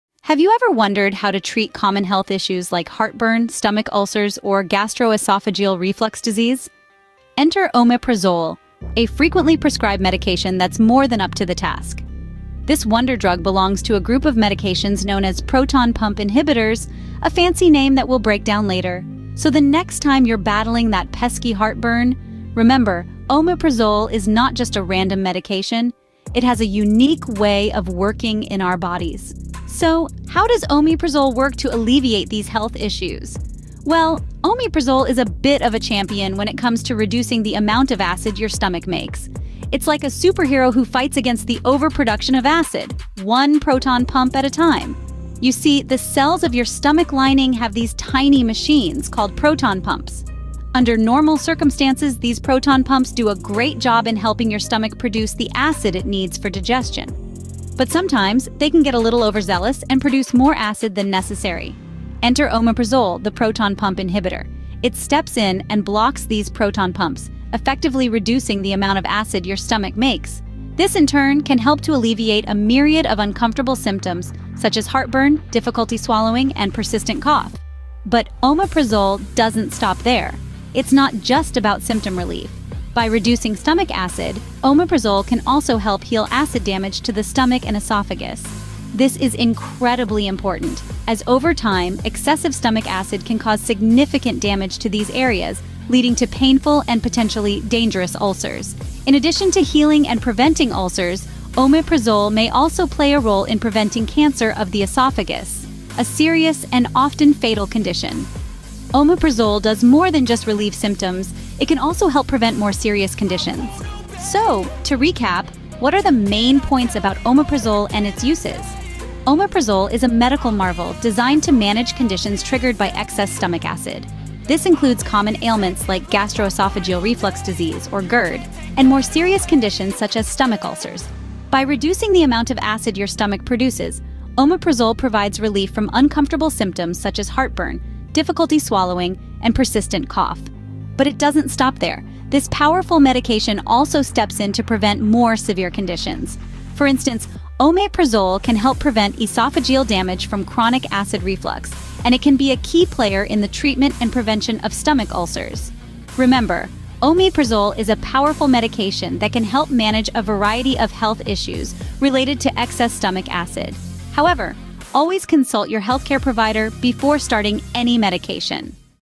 Understanding Omeprazole - Benefits and Medical Applications (3 Minutes)
Curious about Omeprazole and its benefits in medical applications? In this video, we delve into the health benefits and medical uses of Omeprazole, providing insights into its role in treating various conditions. Whether you're a healthcare professional or seeking information for personal knowledge, this comprehensive guide will help you understand the advantages of Omeprazole. Watch now to unravel the medical benefits of this medication!